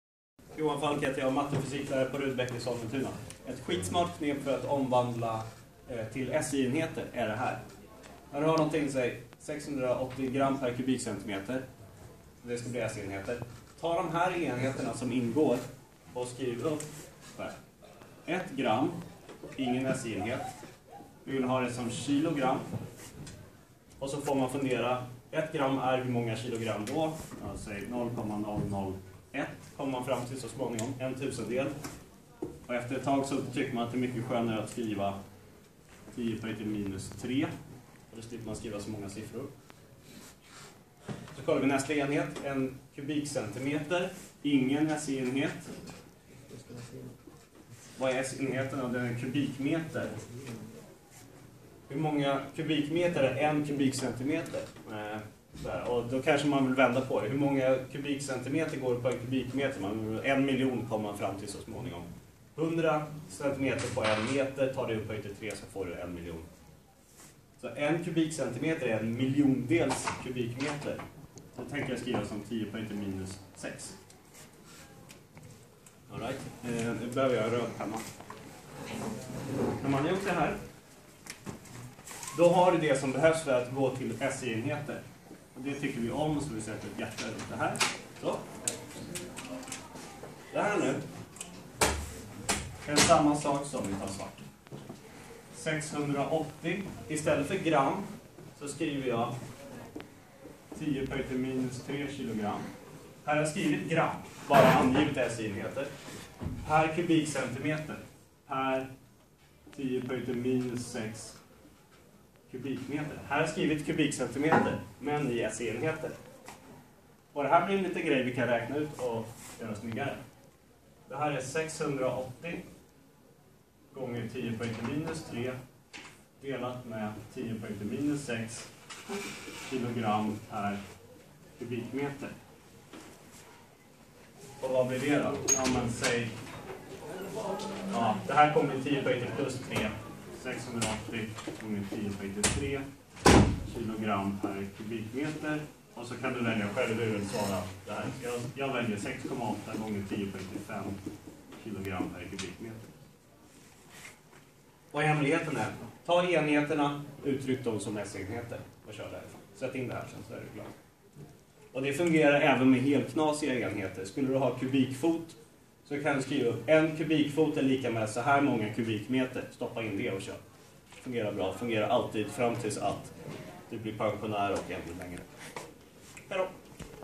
Fysik: smart sätt att omvandla till SI-enheter
Den här videon visar ett fiffigt sätt att omvandla till SI-enheter. Och det är en metod som alltid funkar! (Även till enheter som inte är SI-enheter.)

Tack till Peter som spelade in videon!